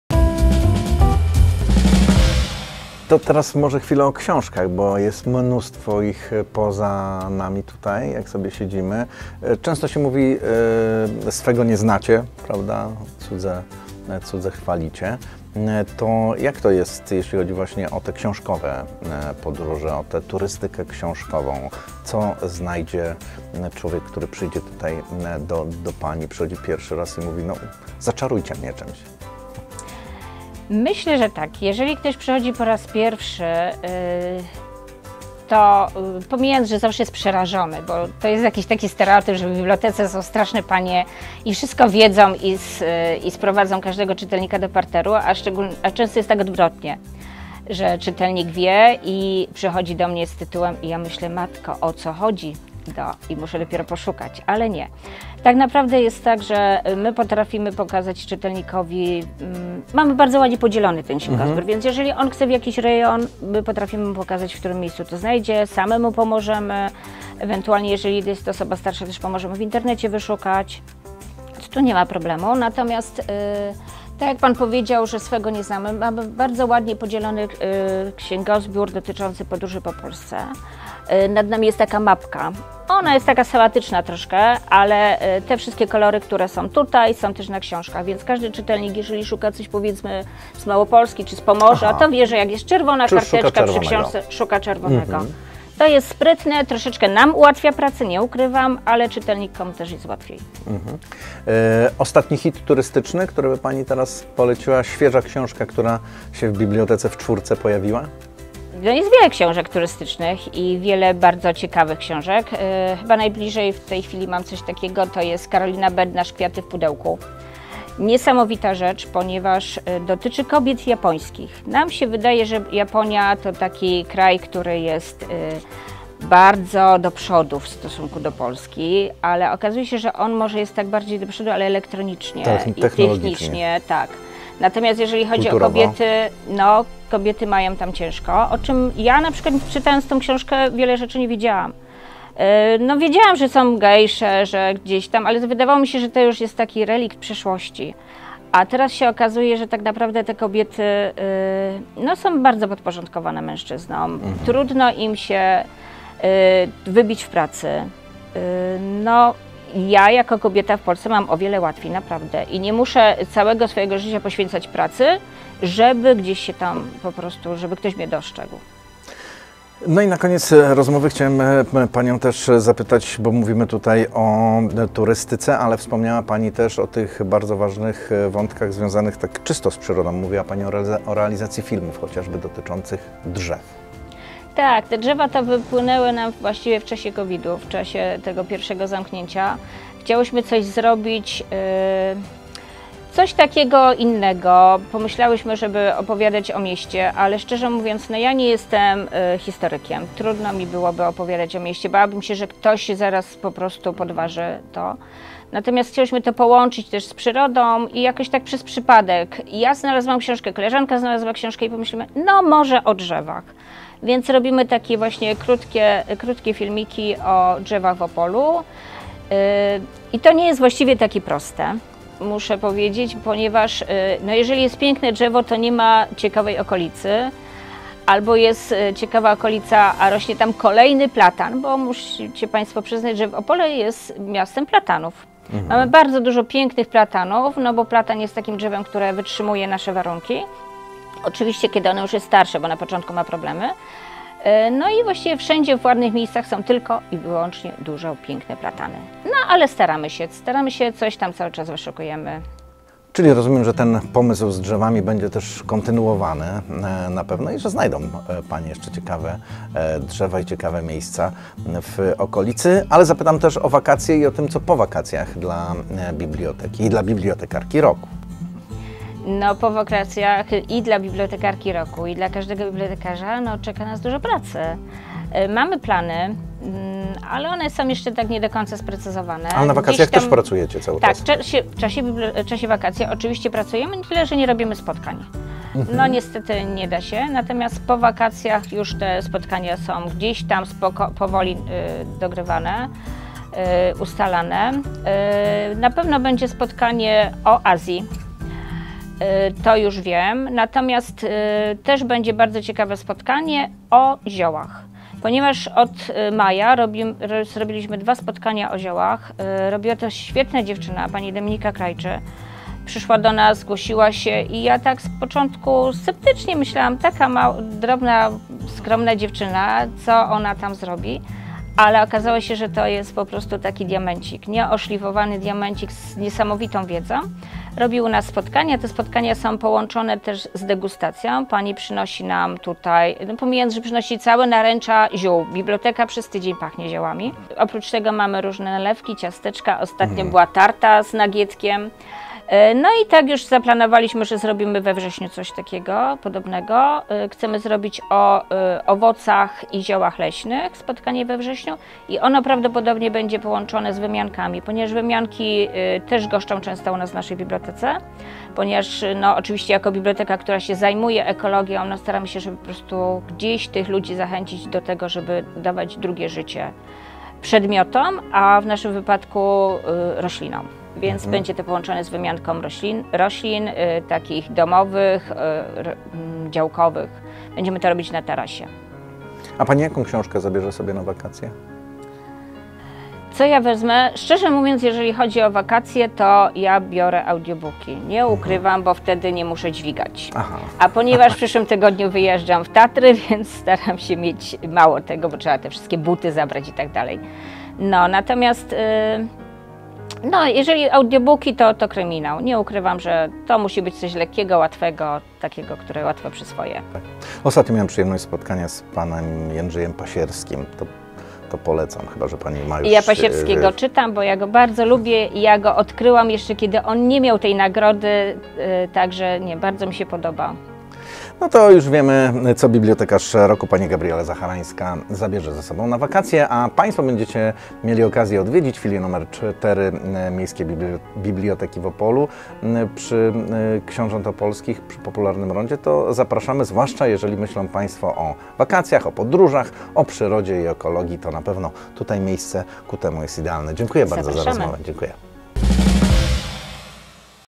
Gabriela Zaharańska Bibliotekarka roku 2021 cz2
Opolski Bibliotekarz Roku 2021 to tytuł, który odebrała niedawno Gabriela Zaharańska - kierownik filii nr 4 Miejska Biblioteka Publiczna w Opolu. Opolanka w ogólnopolskim rankingu uplasowała się na bardzo wysokim 9. miejscu. Z laureatką w dwóch częściach naszego spotkania rozmawiamy o jej pomysłach na "biblioteczne życie". Zapraszamy na część 2.